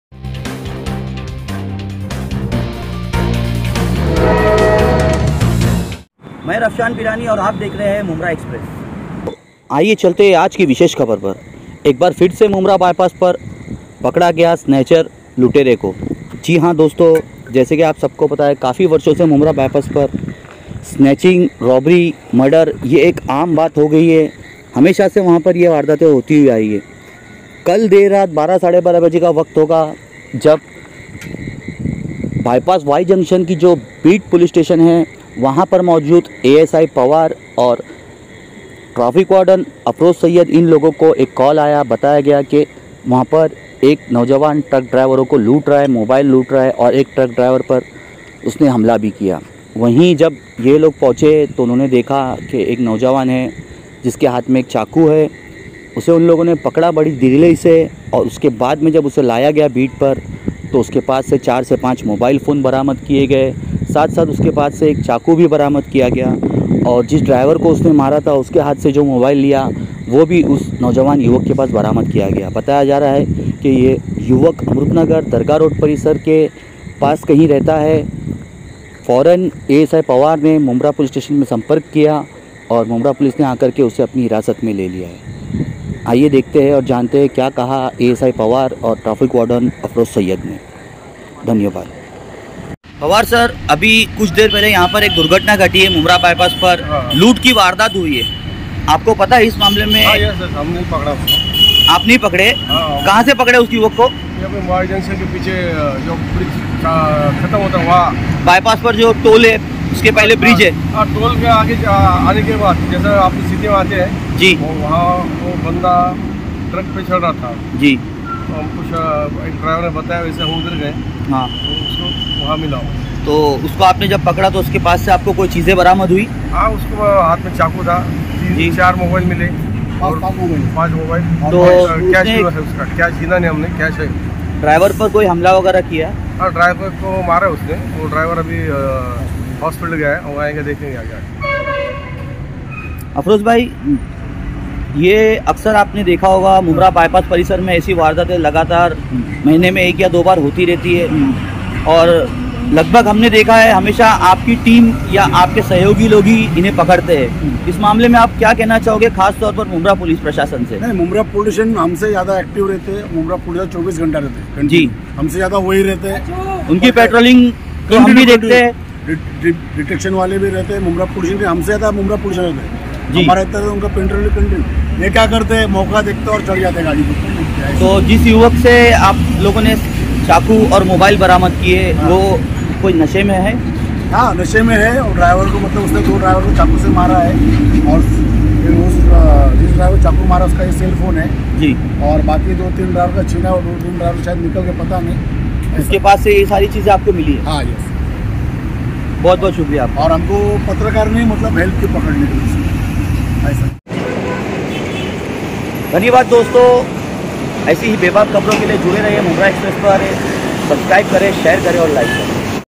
Mumbra Bypaas Pey DER-RAAT Pakda LOOTEREY(SNATCHER)Ko 1-Chaku(🔪)Aur 4-Mobile Ke Sath|Me Tv
Dosto.
Dekhiye Kaisey Ek LOOTERA(SNATCHER)Dey Raha Tha LOOT Ki VAARDAAT Ko ANJAAM...
Rangey Hathon Pakda Gaya Bypaas Ka LOOTERA...

Produced by : Mumbra eXpress News
Edited by       : Rafshan Pirani
On screen      : Rafshan Pirani
Alert viewer :